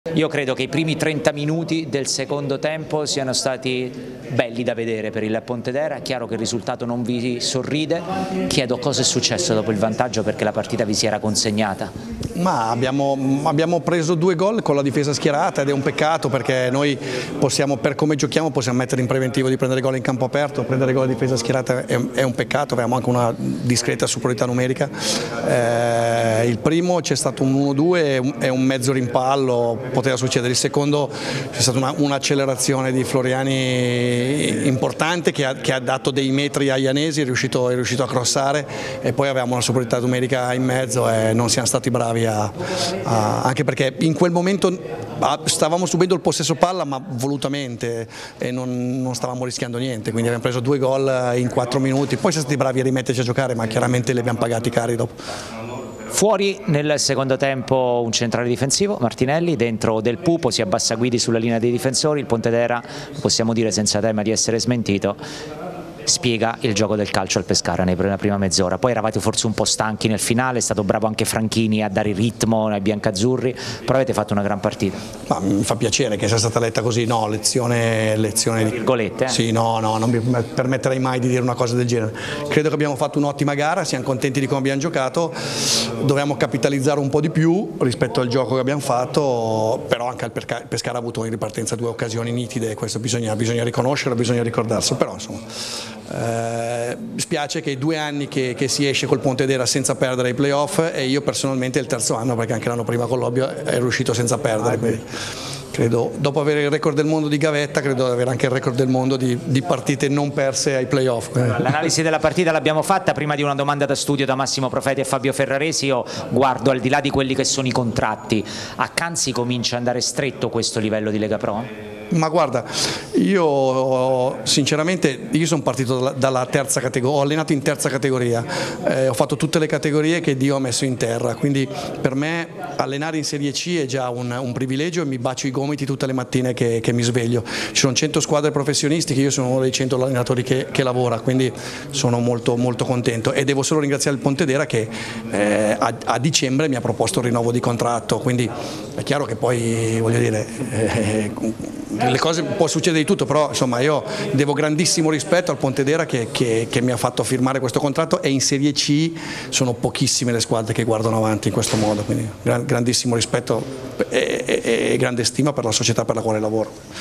Pescara - Pontedera 2-2 Canzi: "Peccato subire gol a difesa schierata"
1° Turno Playoff Girone B Lega Pro